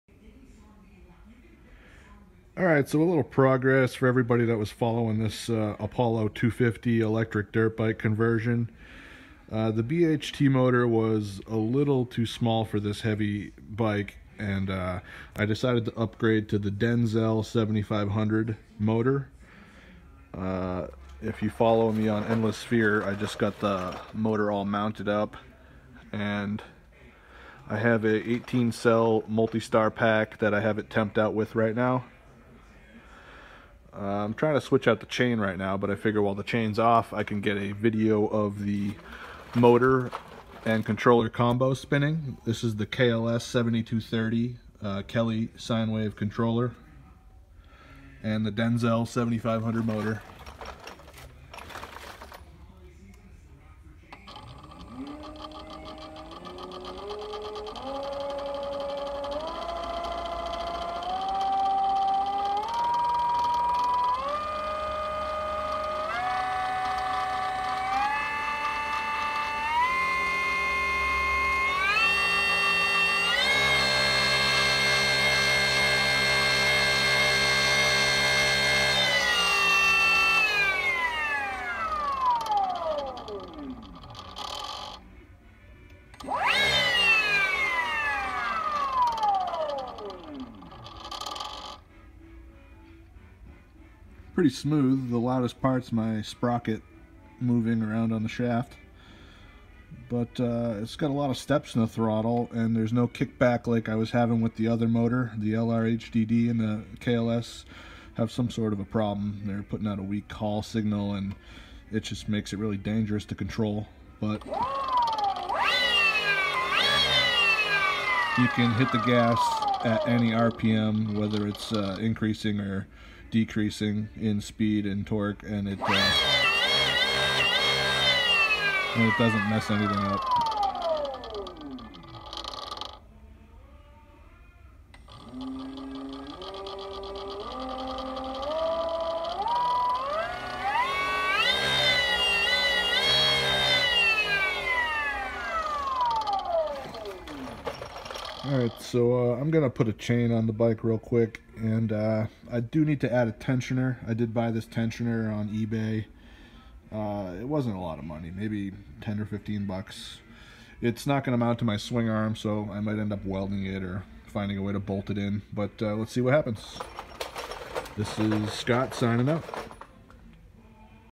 Apollo 250 Electric Dirt Bike Conversion D7500 Test
Upgraded to the 7500w Denzel motor and it works really smooth with the KLS controller